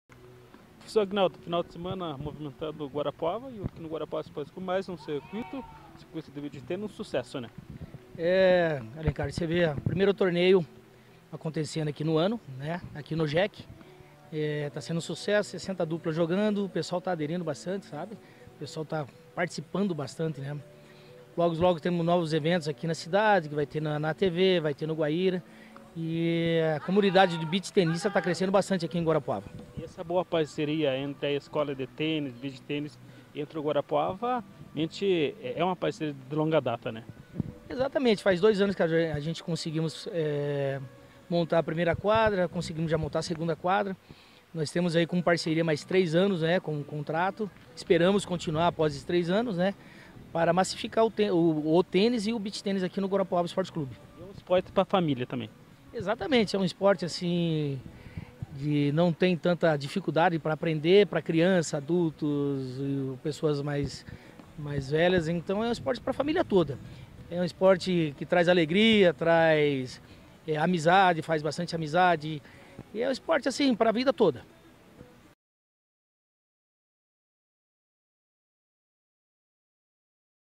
Torneio de Beach Tennis no Guarapuava Esporte Clube
O final de semana está sendo agitado nas quadras de Beach Tennis no Guarapuava Esporte Clube, isso porque mais de 60 duplas estão participando do torneio de Beach Tennis. Acesse Blog//alencarsouza.blogspot.com.br